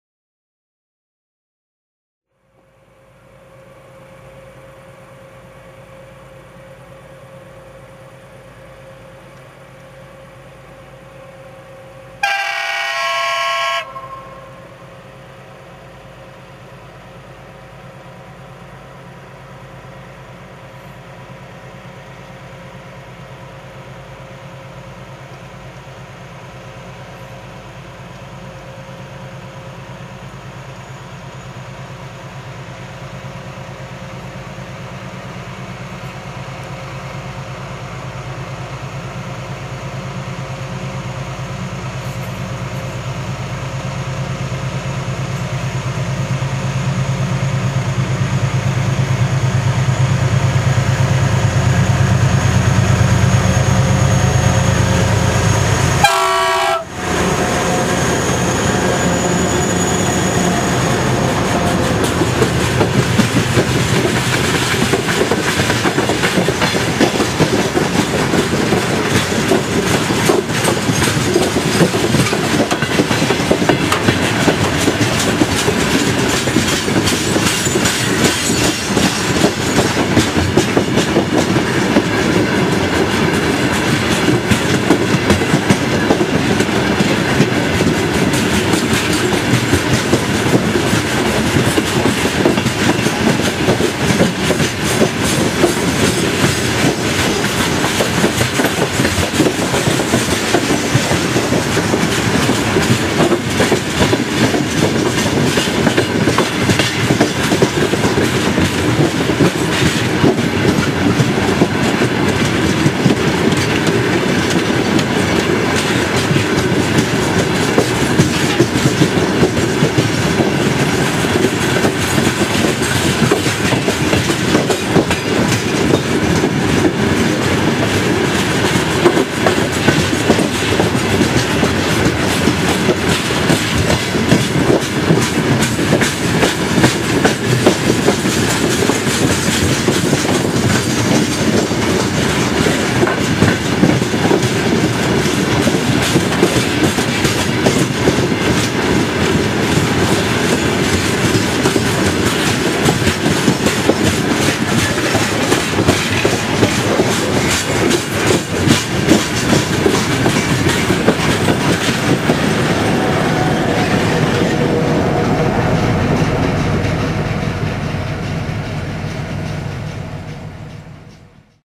[LHS] ST40s-08 + ST40s-01 + ST44-2053 (Rp1)
Batmany z Gagarinem na popychu wloką się z węglarkami w kierunku Zawady.